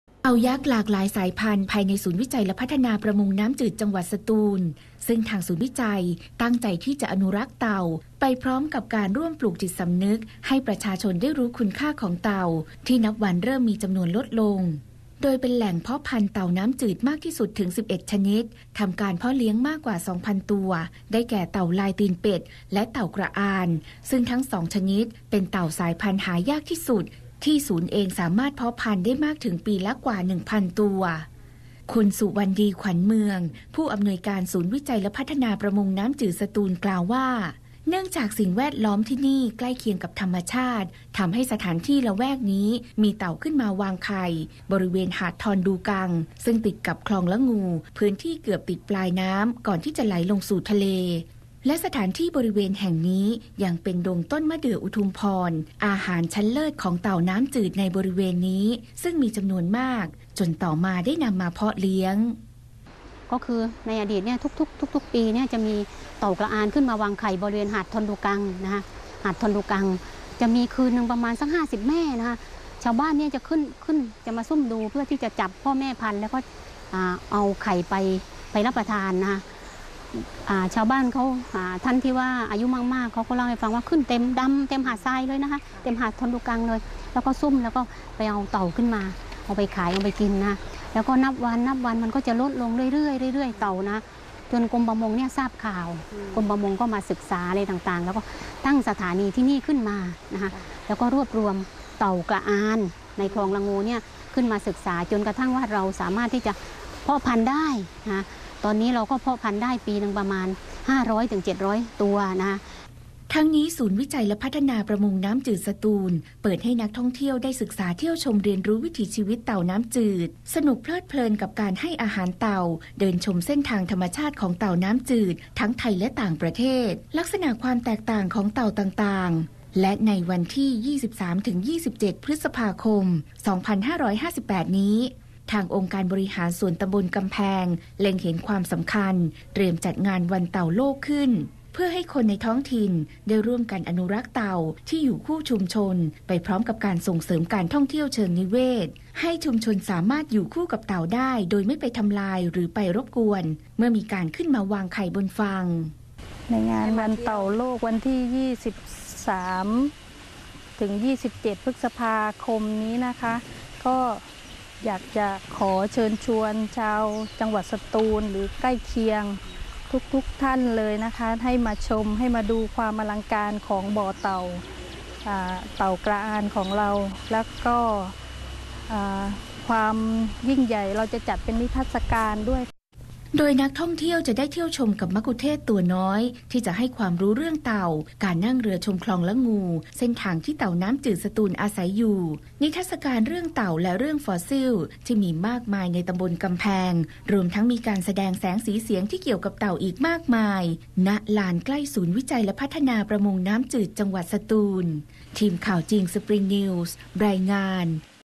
บ้านเพาะพันธุ์เต่าหายาก หลังสุดท้ายในอาเซียน - Springnews
ศูนย์วิจัยและพัฒนาประมงน้ำจืด จ.สตูล ให้การเพาะพันธุ์เต่าน้ำจืดหลากหลายชนิด มากกว่า 2 พันตัวโดยสามารถเพาะพันธุ์ได้มากที่สุดในกลุ่มประเทศอาเซียน ติดตามรายละเอียดได้จากรายงาน